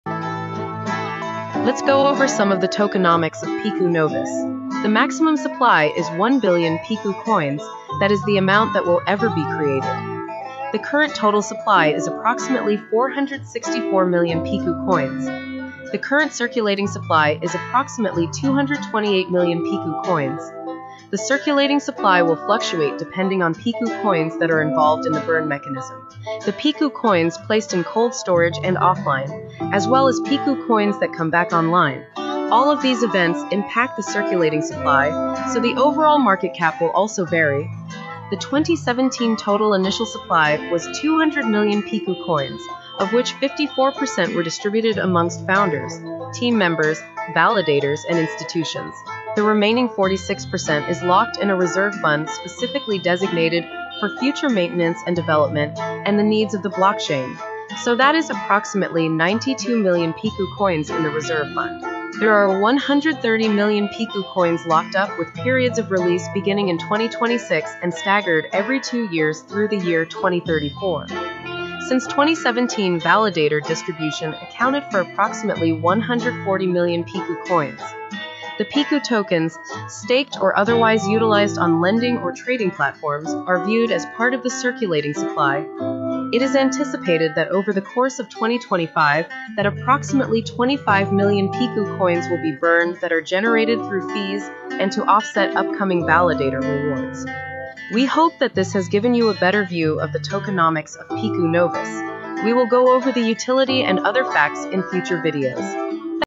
Pecu Novus Blockchain Tokenomics Overview
This is an overview of the tokenomics of the Pecu Novus blockchain, we hope this gives a clearer picture of the mechanics and long term growth potential of the network.